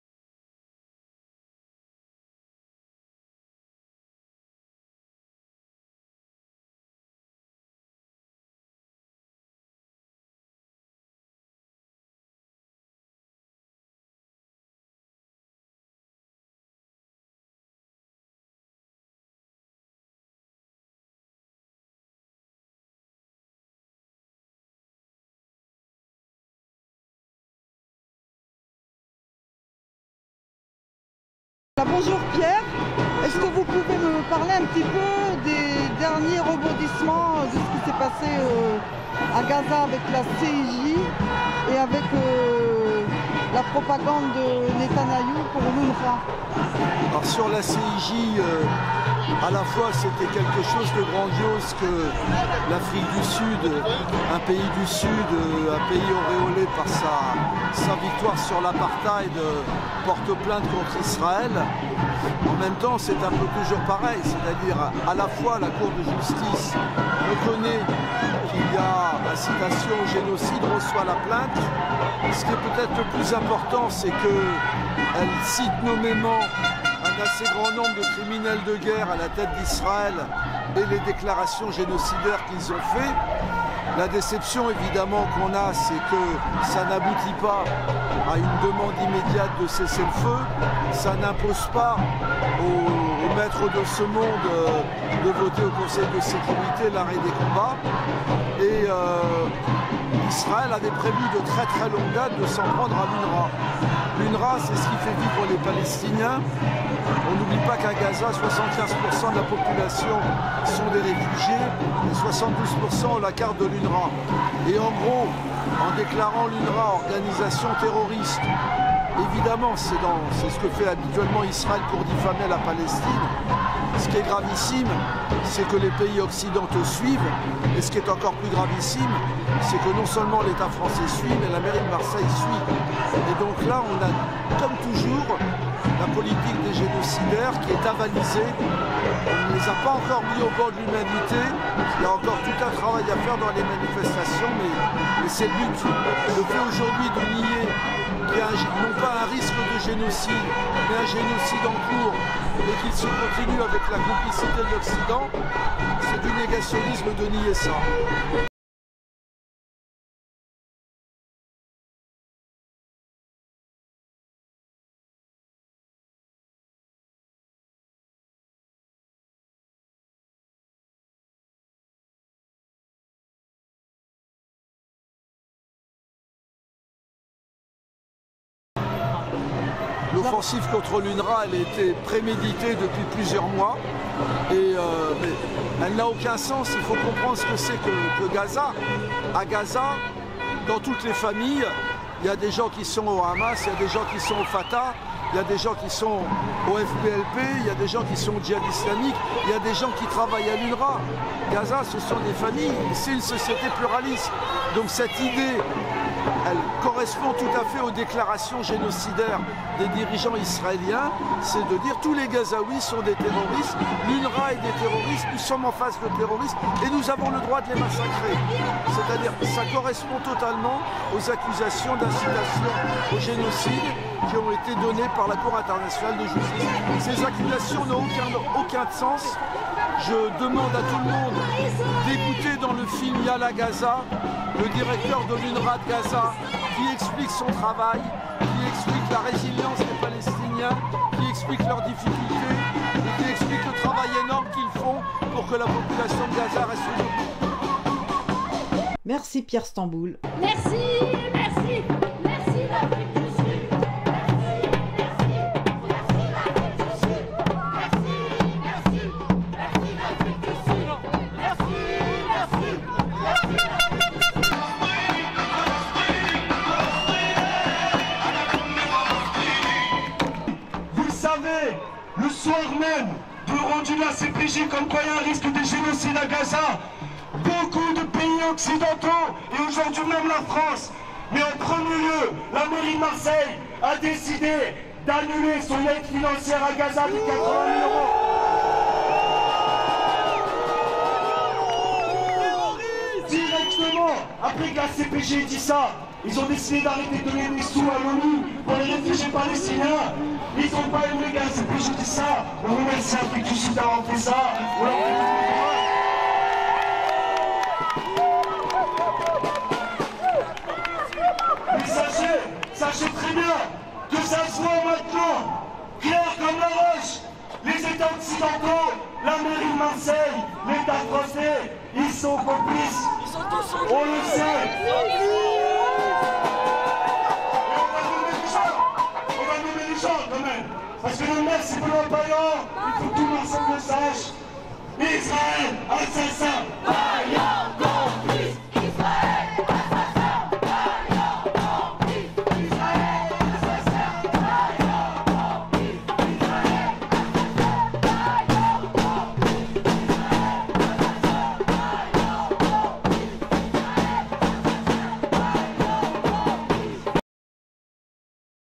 Pierre Stambul: CIJ, Israël, Afrique du sud et soupçon sur UNRWA, génocide en cours..Marseille
Urgence Palestine Marseille
Porte d'Aix à 14h
📢 Manifestation ce dimanche 4  février!

Marseille
Manifestation du 28 janvier 2024 à l'appel de nombreuses organisations pour demander un Cessez le feu à Gaza et l'application des mesures préconisées par la cour Internationale de Justice qui demande de laisser passer l'aide humanitaire et qui reconnait les traces de génocide qui seront examinées dans le futur. en attendant le gouvernement israélien n'ayant jamais respecté le droit international trouve le moyen de criminaliser l'agence de l'ONU d'aide aux réfugiés palestiniens UNWRA réfugiés dans leur propre pays et aussitôt tous les occidentaux et la mairie de Marseille avant tout le monde décident de ne plus verser d'aide en attendant que la situation d'accusation de 12 personnes de l'UNWRA ayant soi disant participé à 'attaque du 7 octobre soient jugés.Bien entendu les accusations viennent du gouvernement israélien.
Dans cette vidéo je demande des explications à Pierre Stambul de l'UJFP: Union juive française pour la Paix.